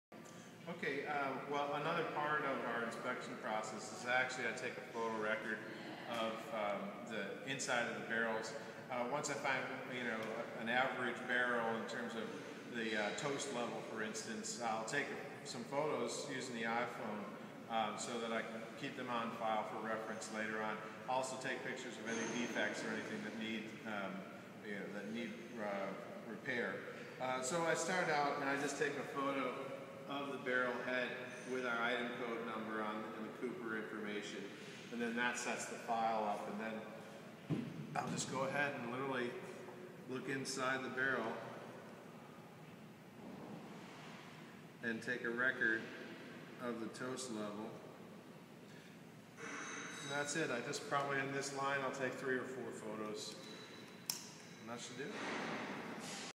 Using the iPhone as a Wine Barrel Inspection Tool | Winemaking Process Videos
Cellarmaster Patrick Fallon uses his iPhone to inspect new wine barrels, creating records of each oak barrel, inside and out, received at Jordan Winery. Inspecting the quality of each barrel is critical to he winemaking process.